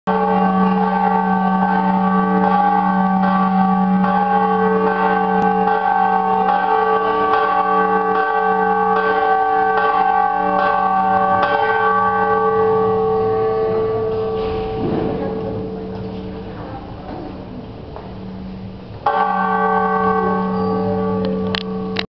CARDENAS CHURCH BELLS
Man ringing the bells in the main Catholic church near the main plaza of Cardenas.